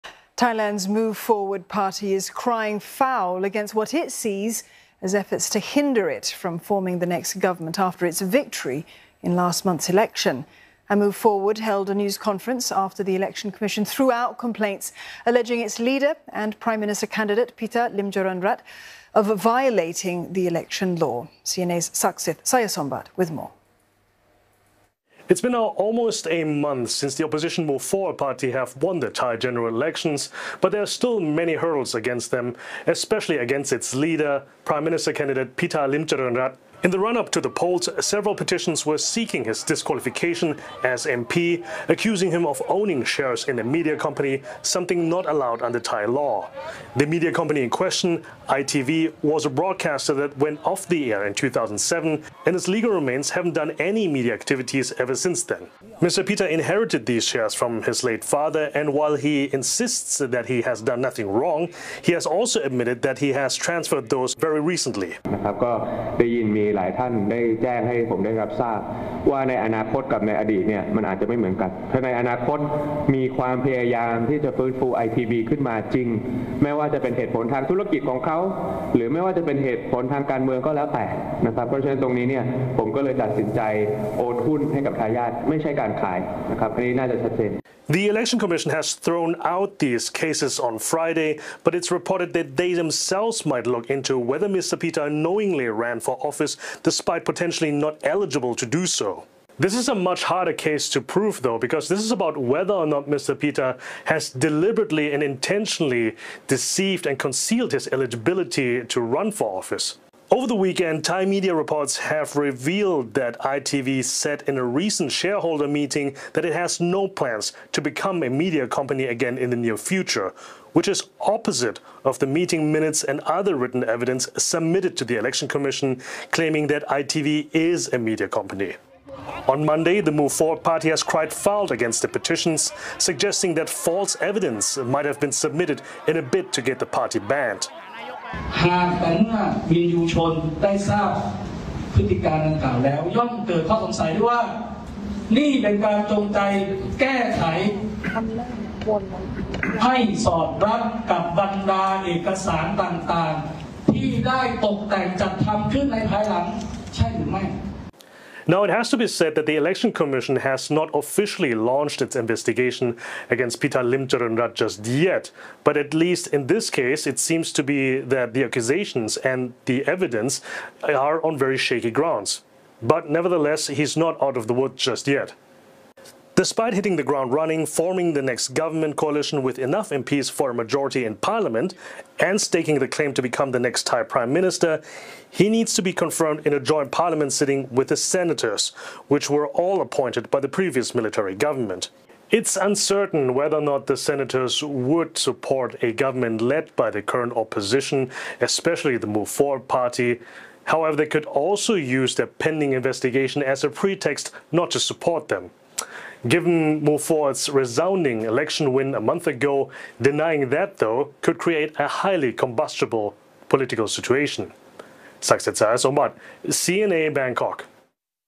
Thai election commission to probe PM frontrunner Pita
Thailand’s Move Forward Party is crying foul against what it sees as efforts to hinder it from forming the next government, after its victory in last month’s election. It held a news conference after the Election Commission threw out complaints alleging its leader and prime minister candidate Pita Limjaroenrat had violated the election law.